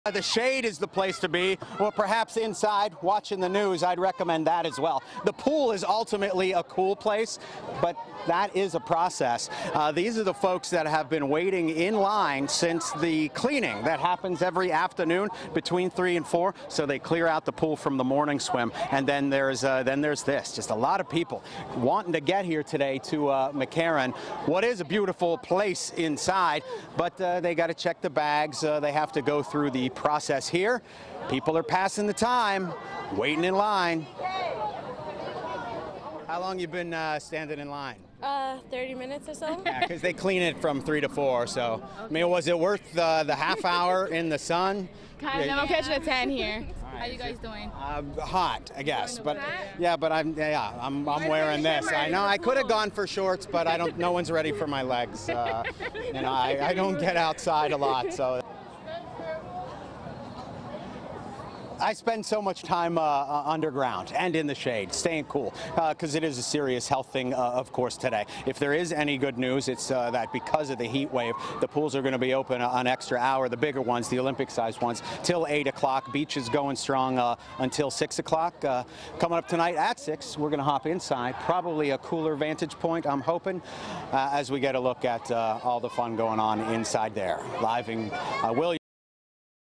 First heat wave of the year scorches NYC
Temperatures Monday once again climbed into the 90s, marking an official heat wave in New York City with no clear end in sight.